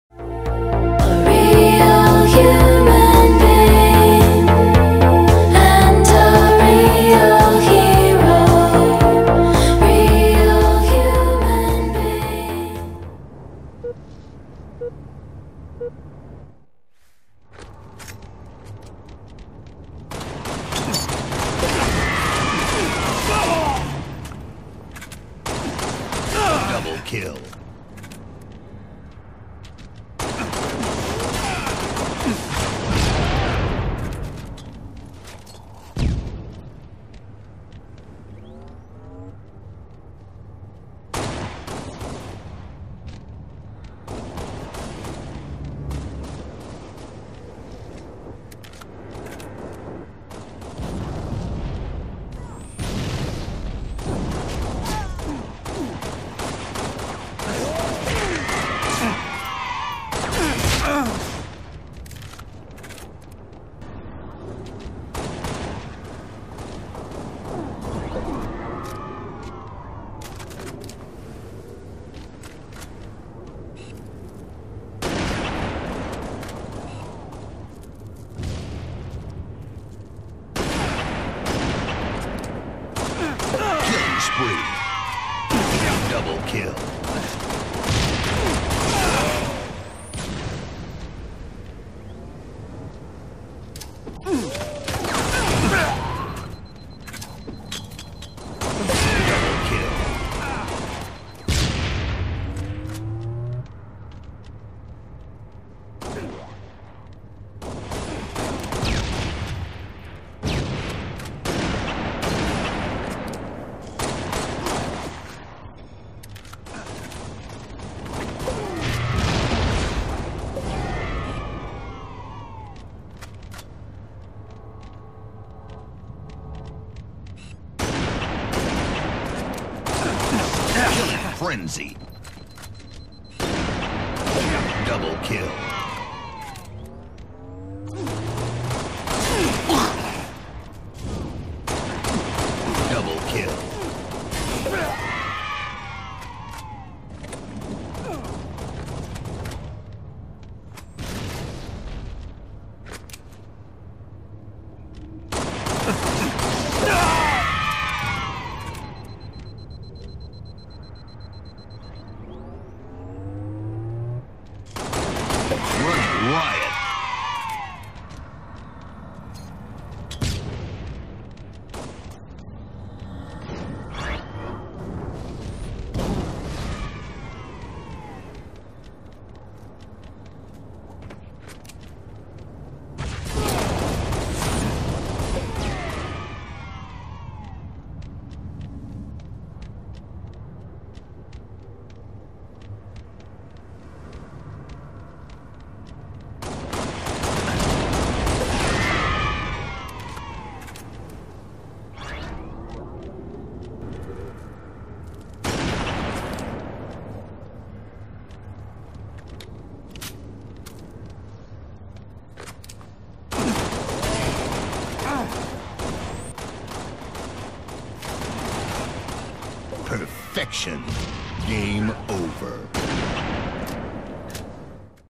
FuzzylilPeachh :: Anniversary Perfection
Boring 17-0 gameplay with some alright doubles.
Still accepting clips for the anniversary top ten, clips can be anything at all